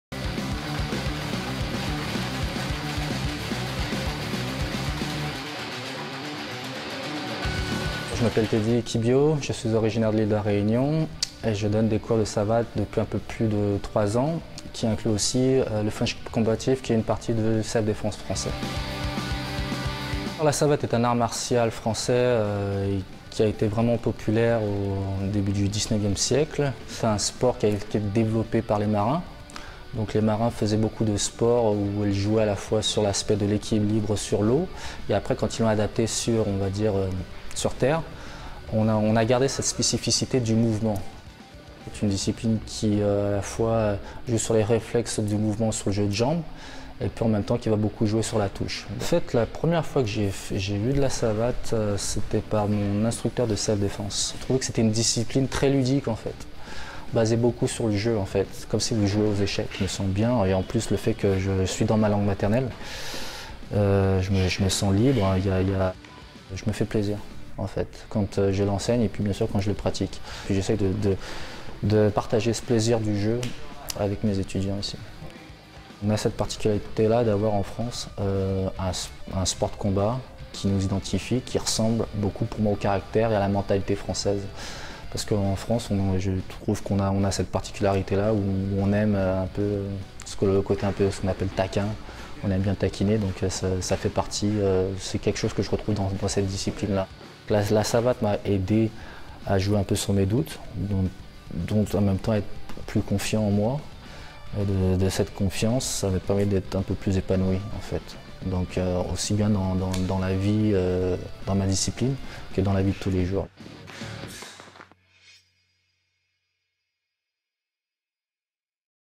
Fureteur : arts martiaux – la savate
Teddy Kybio est originaire de La Réunion. Depuis 3 ans, il enseigne la savate, un art martial français pratiqué par les militaires sous Napoléon. Il nous fait découvrir cette discipline qui occupe une grande place dans sa vie à Vancouver. Notre fureteur Johann Nertomb l’a rencontré.